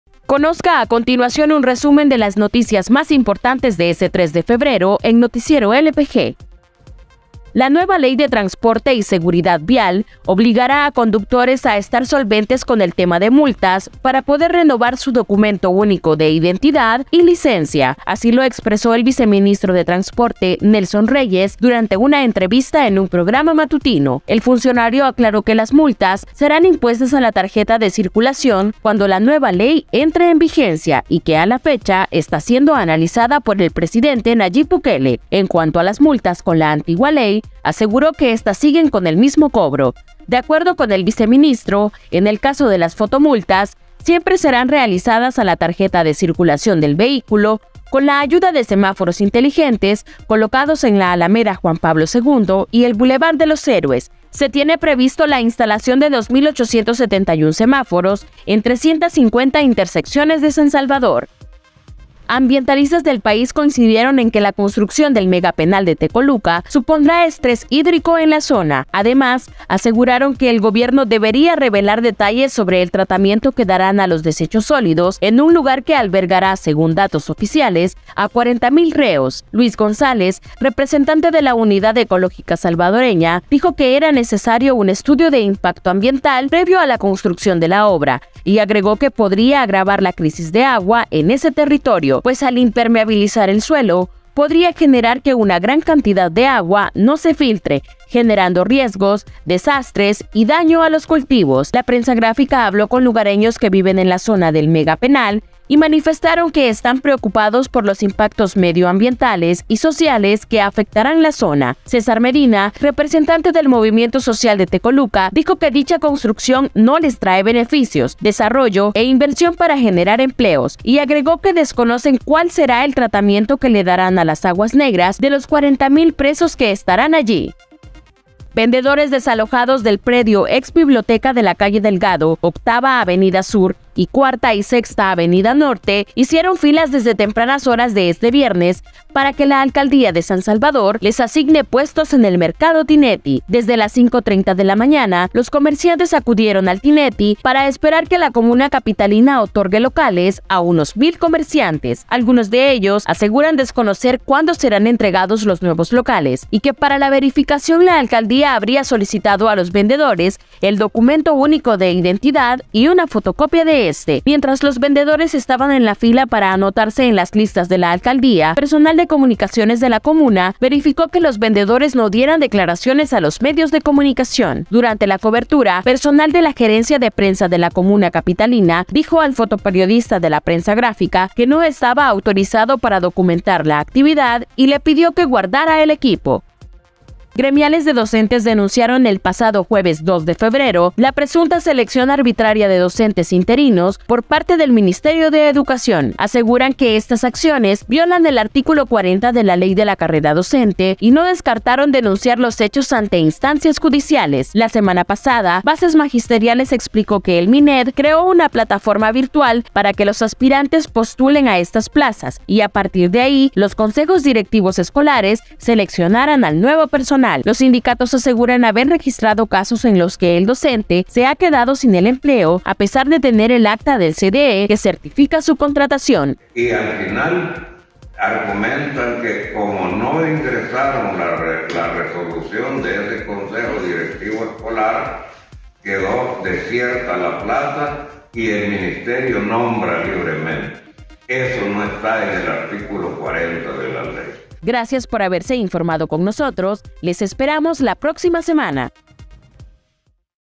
Noticiero LPG 3 de febrero:Conductores deberán estar solventes de multas para renovar DUI y licencia
La nueva ley de transporte y seguridad vial obligaría a conductores a estar solventes con el tema multas para poder renovar su Documento Único de Identidad y licencia.